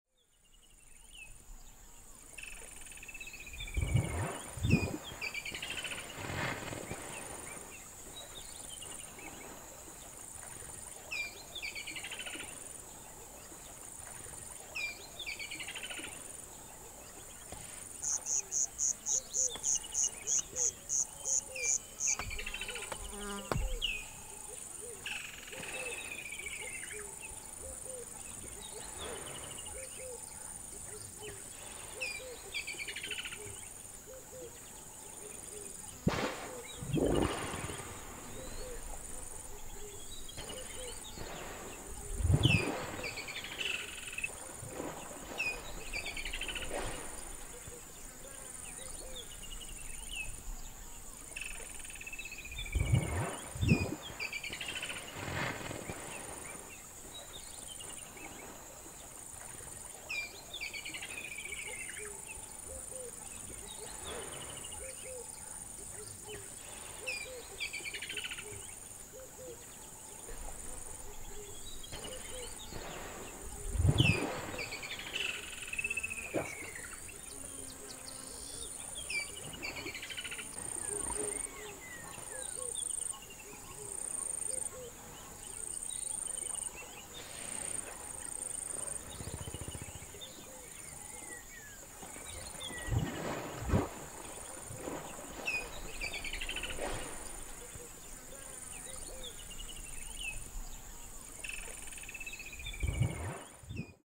Hippopotami Approaching - 8K clip
A pair of Hippos walking toward me along a channel in the Okavango Delta, Botswana. The Hippopotamus in the lead notices me filming from the bank and raises his head to get a better look. Shot on Red Helium in 8K.